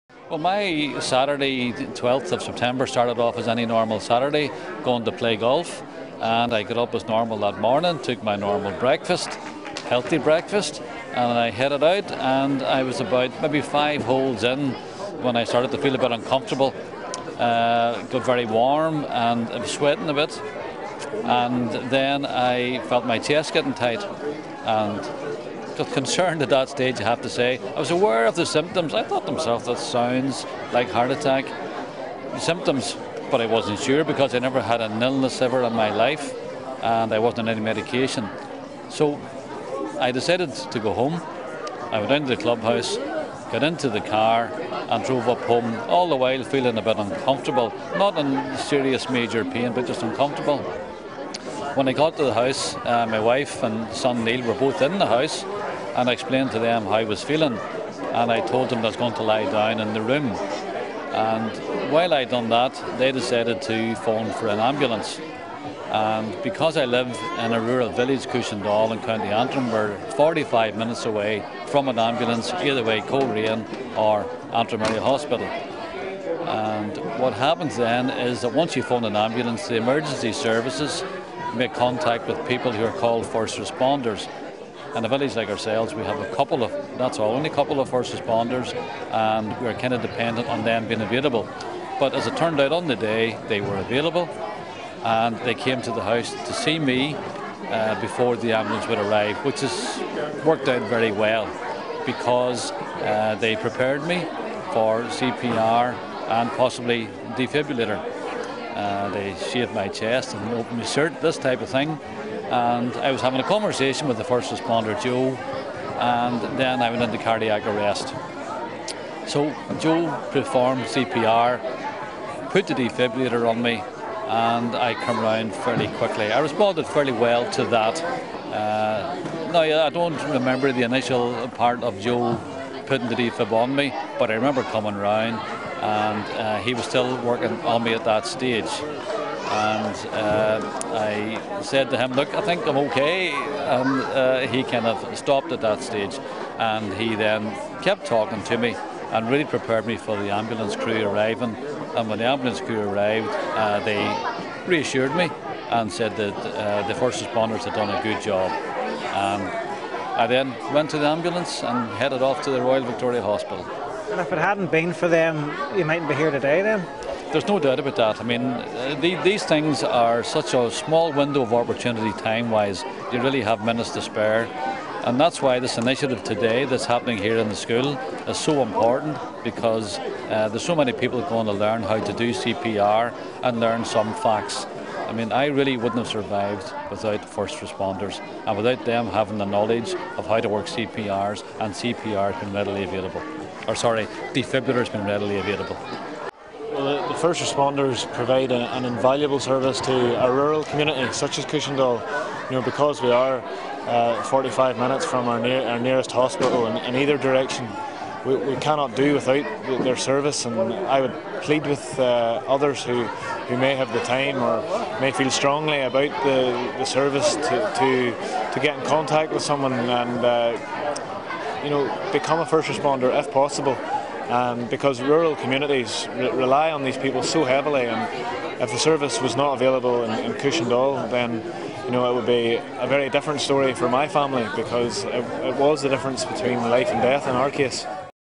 First responders saved my life: Co Antrim man
A Co Antrim man has told of how Community First Responders (CFPs) saved his life after he had a cardiac arrest.

Hugh McManus from Cushendall, on the north Antrim coast, says he is indebted to the local CPRs who kept him alive until paramedics arrived.

Cushendall is 45 minutes from the nearest hospital.

He was speaking at St Louise's Comprehensive in west Belfast, one of a number of NI schools taking part in a UK-wide initiative to train young people in basic cardiopulmonary resuscitation skills (CPR).

The programme is being organised locally by the Northern Ireland Ambulance Service in association with the British Heart Foundation and other associated organisations.

Throughout Northern Ireland, 1400 people suffer out-of-hospital cardiac arrest each year - but less than 10% survive.